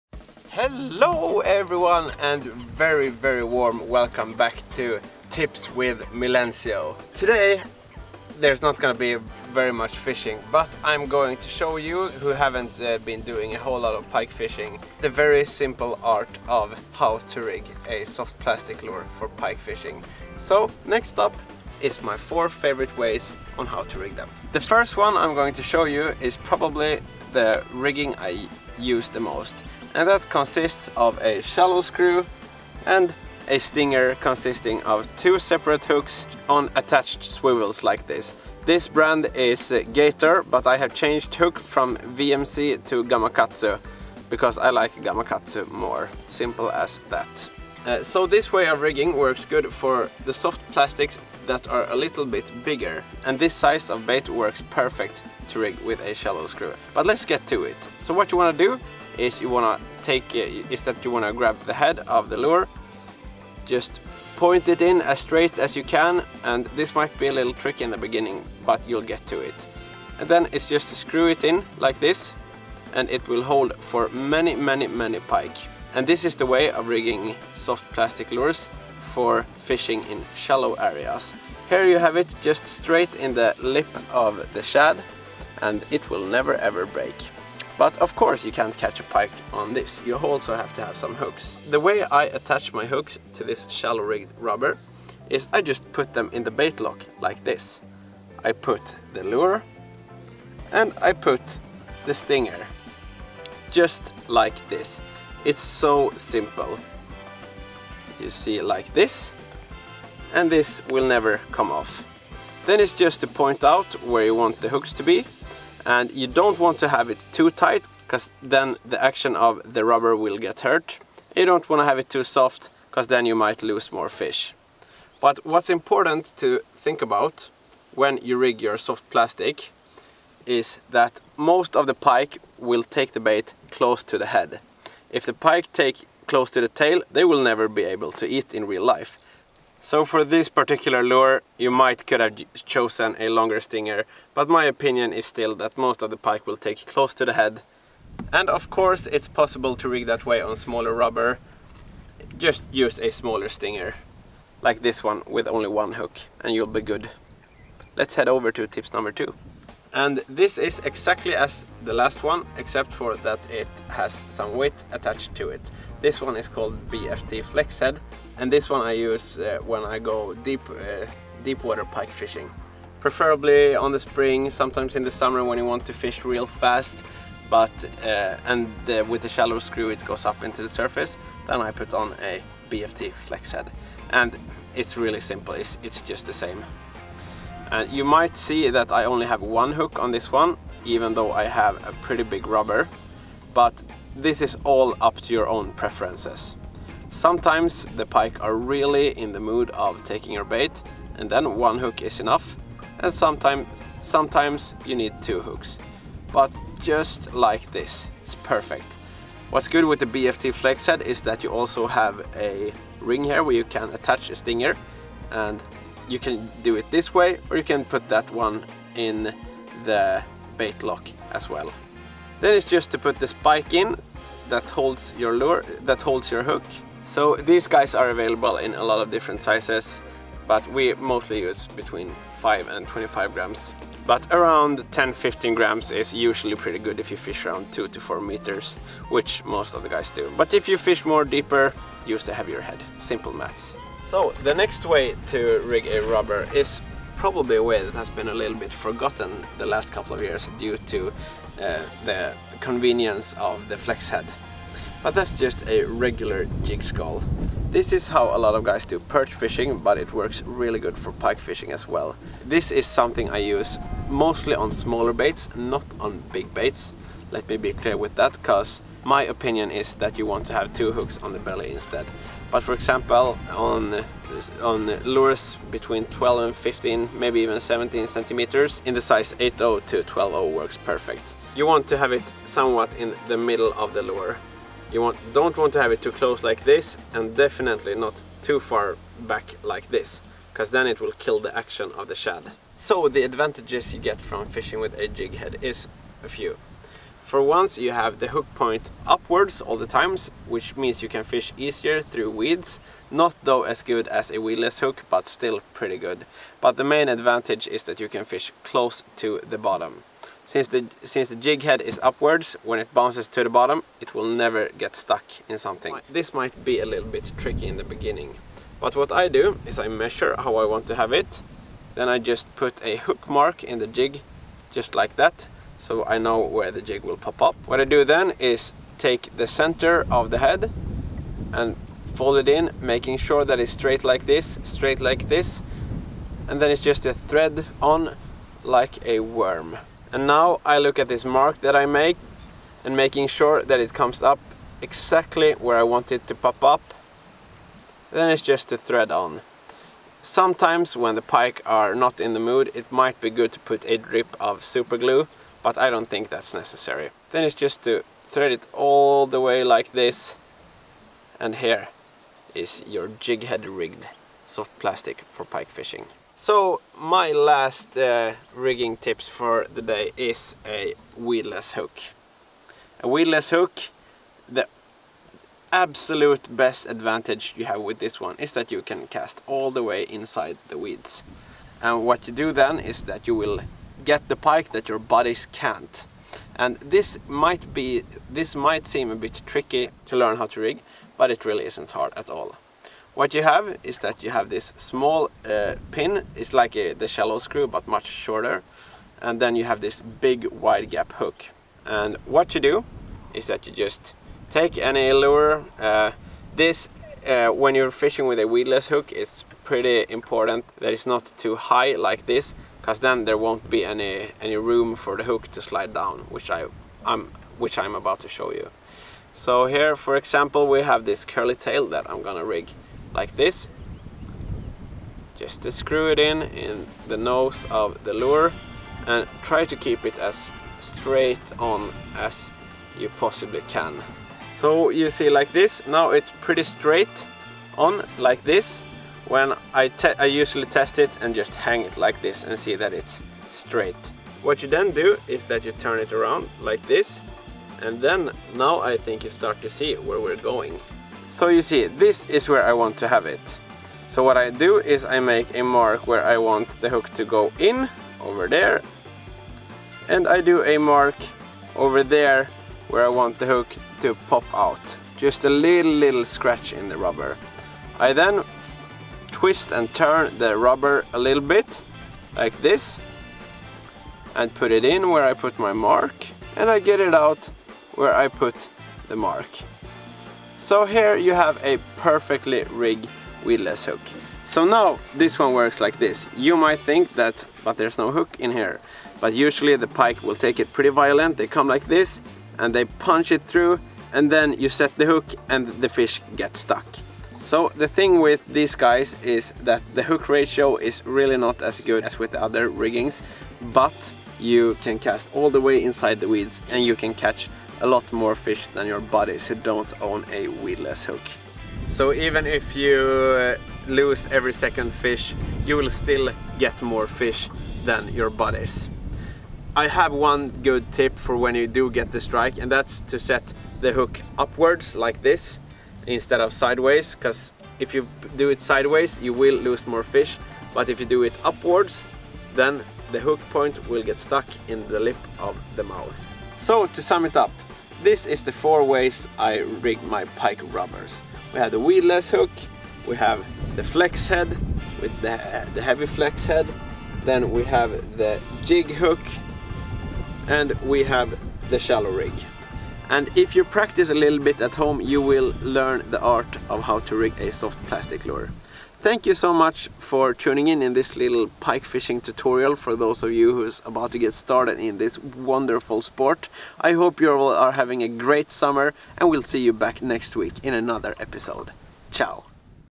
How to rig a PIKE SOFTBAIT (Shallow, Weedless, Jighead & Flexhead)
Softbaits, rubber baits, jigs - whatever you call them, these fish-catching machines can be rigged in a variety of ways and in this video Mille will walk you through 4 different ways that they can be rigged. All depending on if you want to fish, deep, shallow, fast, slow, inside the weed or anywhere else...